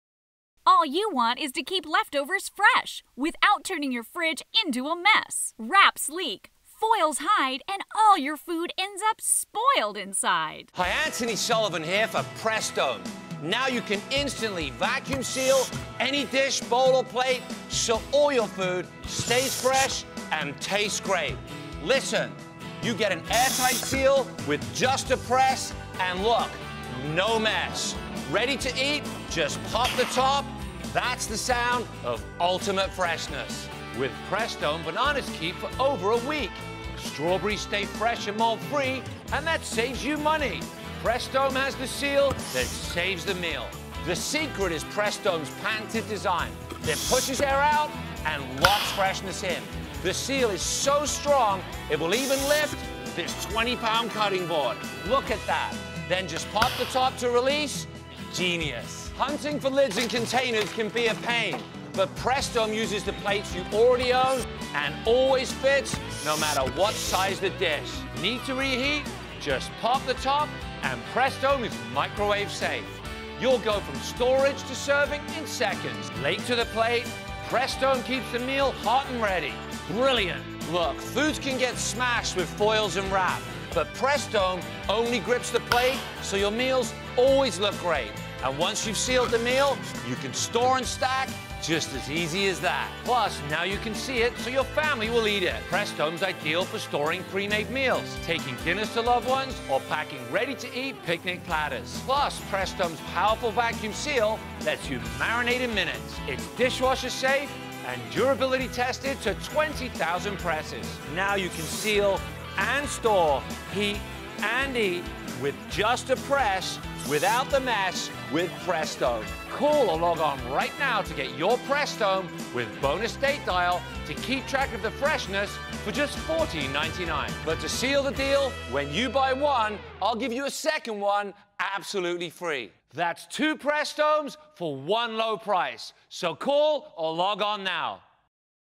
Press Dome™| Official Commercial | Top TV Stuff
Unlike the average vacuum food sealer that are a hassle to use and require expensive equipment, Press Dome™ Vacuum Sealer  has a powerful built-in vacuum pump and seals directly to your own plates, bowls, , even counter tops and cutting boards. Press Dome™ is quick and easy to use.  Just press the Built-In Vacuum Pump to seal, and pull up to open. Use it to quickly vacuum seal leftovers, marinate meat, keep fruit fresh, or to take lunch to work or school, It goes from fridge or freezer, to microwave, to dishwasher, to cupboard, and it's BPA Free!